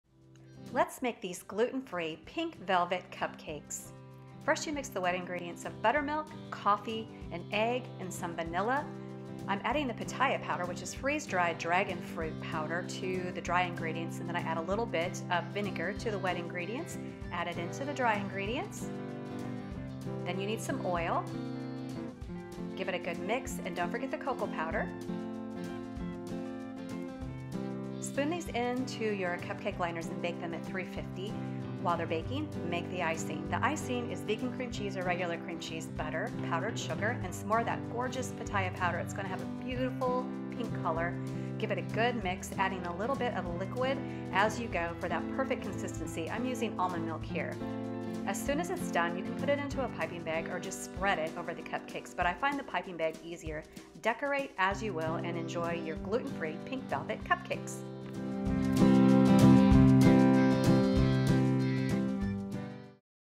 Discover the Perfect Recipe for Gluten-Free Pink Velvet Cupcakes - TKFoodie.com
Easy, chocolatey pink velvet cupcakes (my riff on red velvet) with a delectable cream cheese (or vegan cream cheese icing that happens to be beautifully fuchsia pink as well. Check out TKFoodie.com for the simple recipe which is totally adaptable to be dairy free as well as gluten free.

Cupcake tin
Fun Cupcake liners
Hand mixer
Essential Mixing Bowls
edible Glitter
Pataya (dragonfruit) powder
Piping Bags with tips